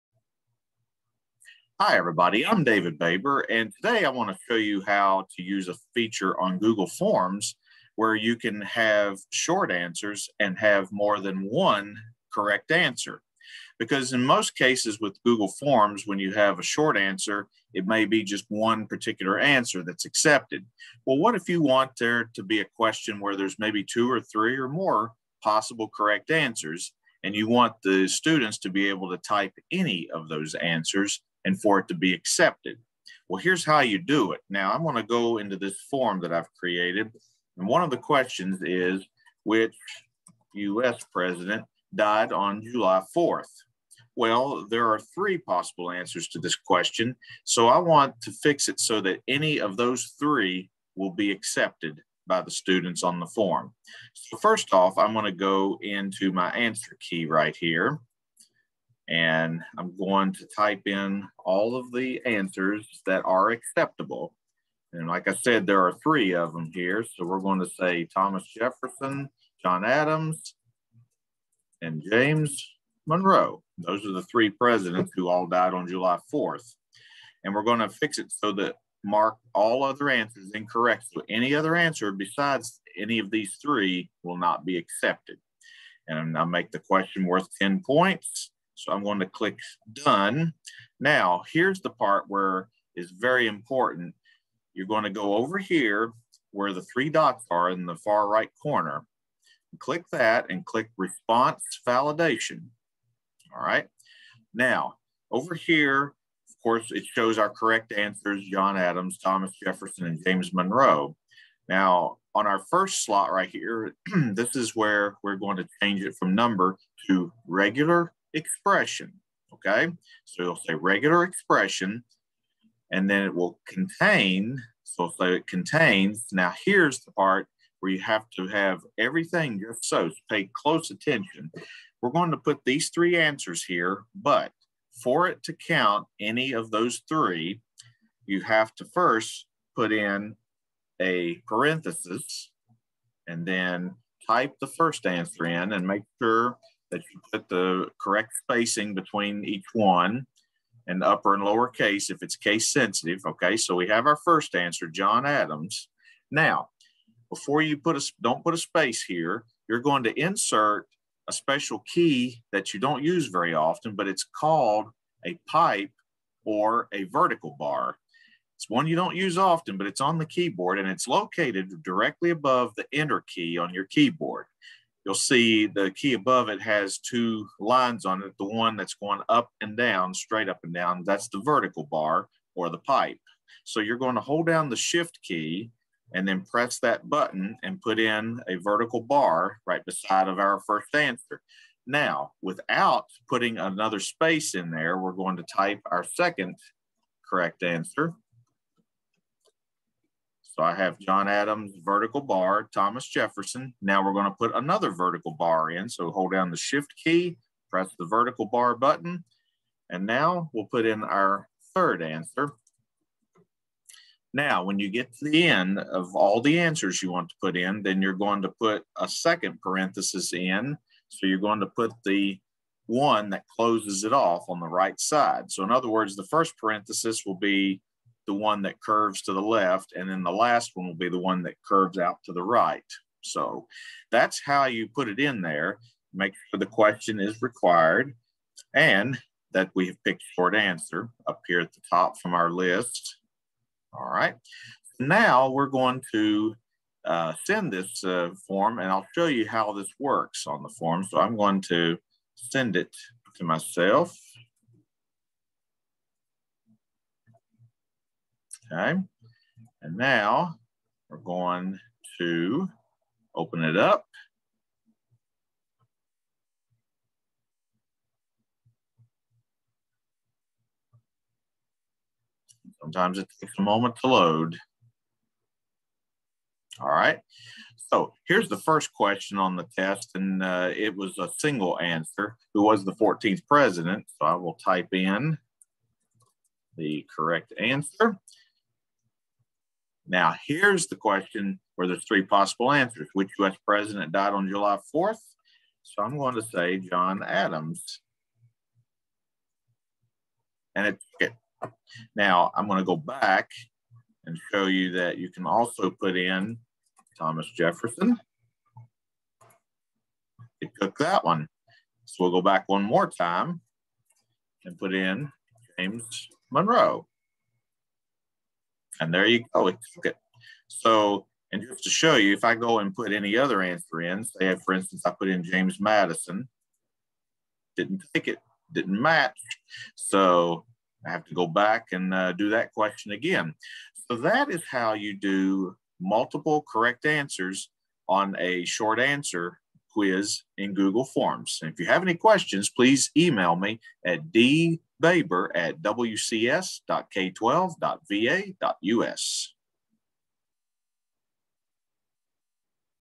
Google Forms Short Answer Multiple Answers Tutorial
This tutorial shows you how to create questions in Google Forms with short written answers and also questions with multiple answers.  I apologize in advance for the audio quality in parts of the video.  My computer microphone was not working too well when I recorded this video.